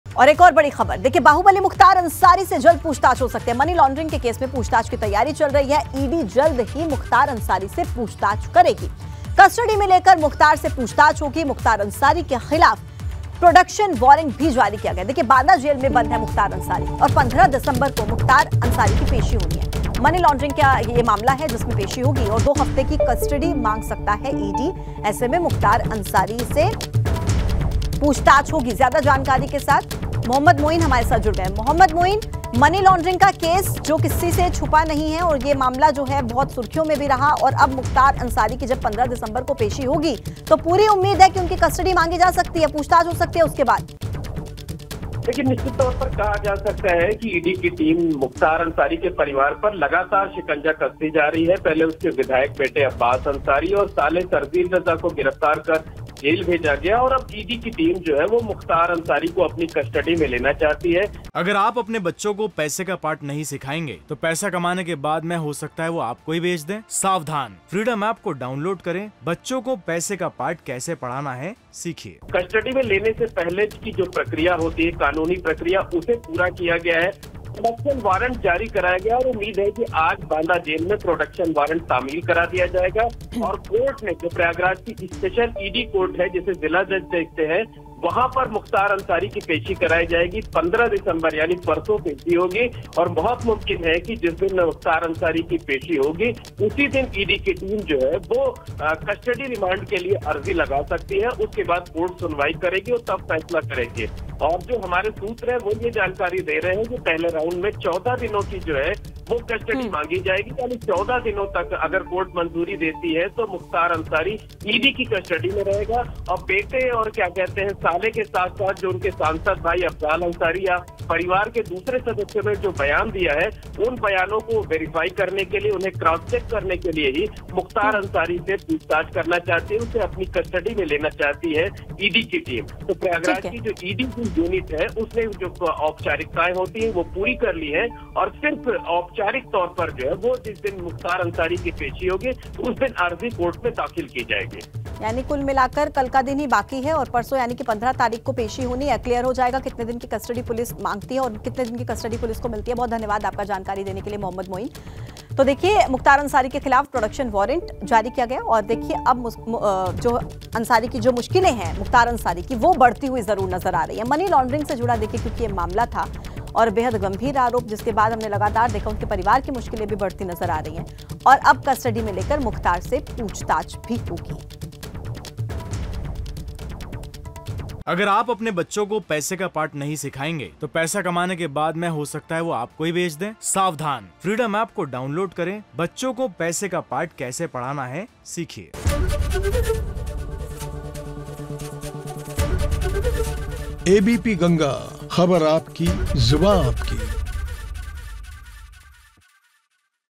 Mukhtar Ansari की फिर बढ़ेंगी मुश्किलें, मनी लॅान्ड्रिंग केस में ED करेगी पूछताछ
Mukhtar Ansari की फिर बढ़ेंगी मुश्किलें, मनी लॅान्ड्रिंग केस में ED करेगी पूछताछ, ED मुख्तार अंसारी को कस्टडी में लेकर करेंगे पूछताछ..देखिए